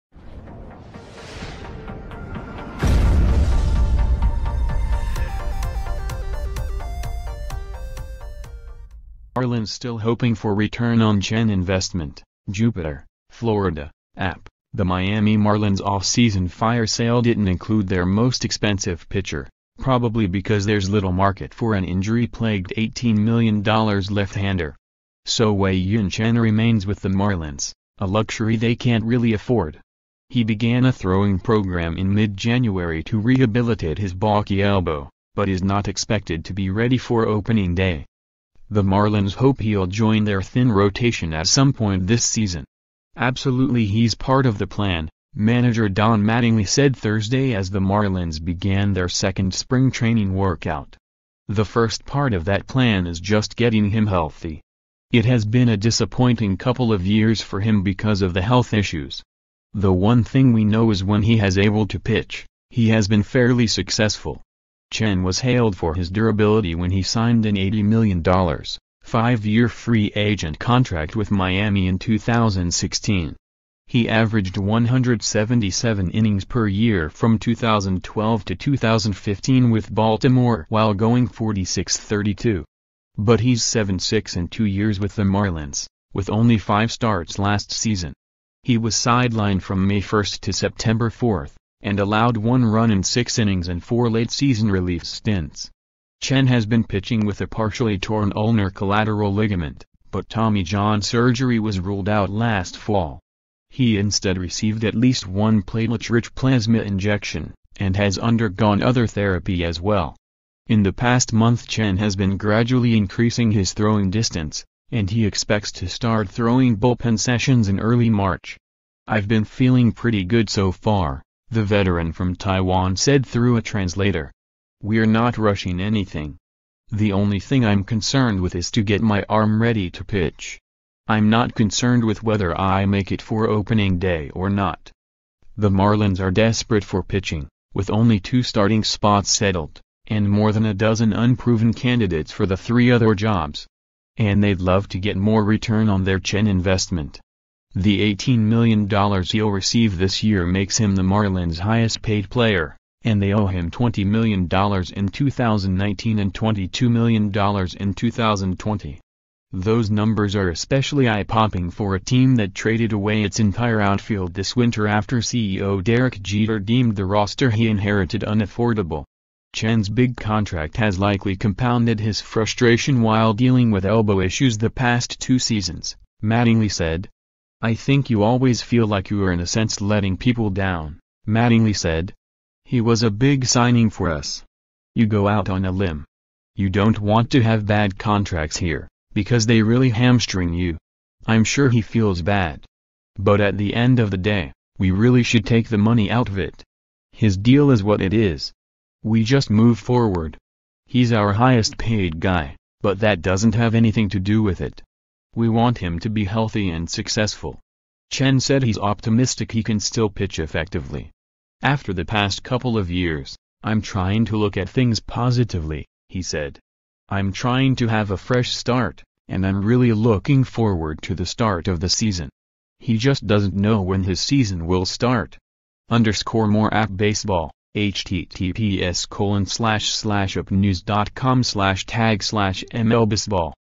Marlins still hoping for return on Chen investment
JUPITER, Fla. (AP) — The Miami Marlins' offseason fire sale didn't include their most expensive pitcher, probably because there's little market for an injury-plagued $18 million left-hander.

So Wei-Yin Chen remains with the Marlins, a luxury they can't really afford. He began a throwing program in mid-January to rehabilitate his balky elbow, but is not expected to be ready for opening day.

The Marlins hope he'll join their thin rotation at some point this season.